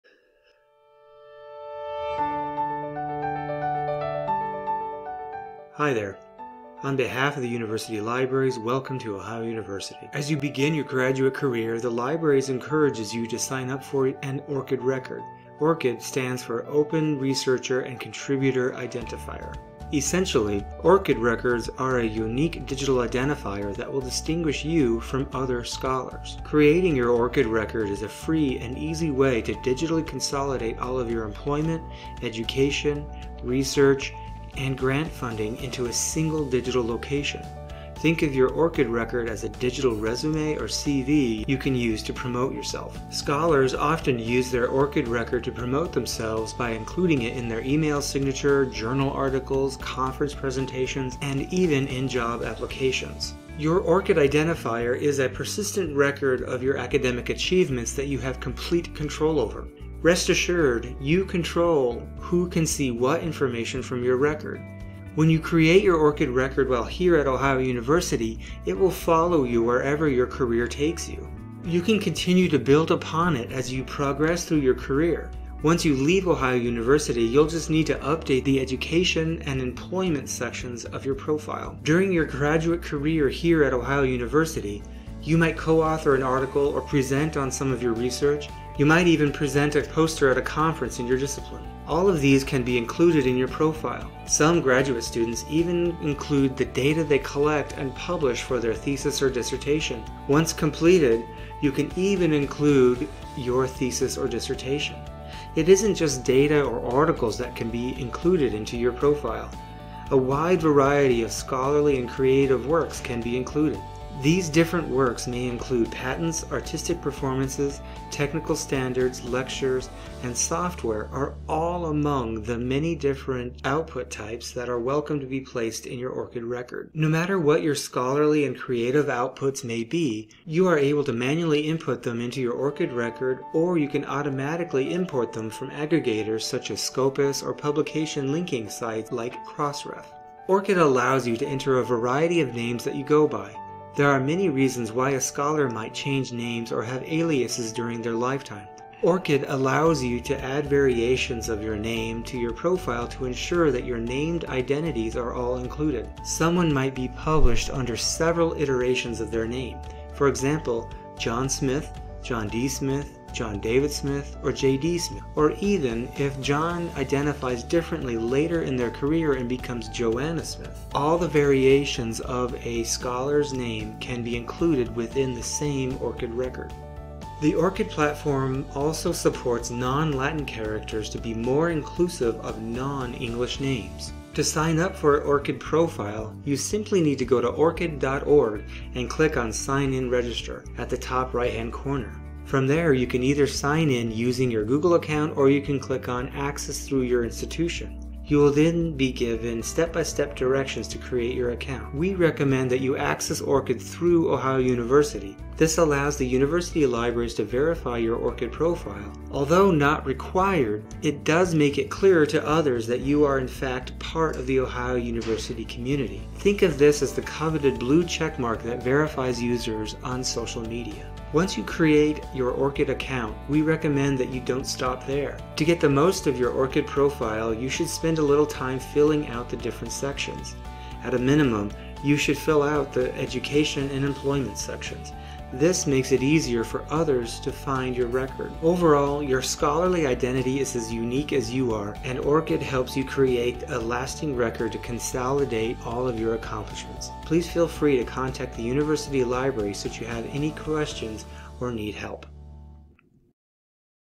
ORCiD for OHIO Graduate Students at OHIO University
This video introduces OHIO graduate students to the many benefits of creating an ORCiD profile. Students will learn how ORCiD can be used to distinguish themselves from other scholars, promote themselves, and to curate their own digital scholarly identity.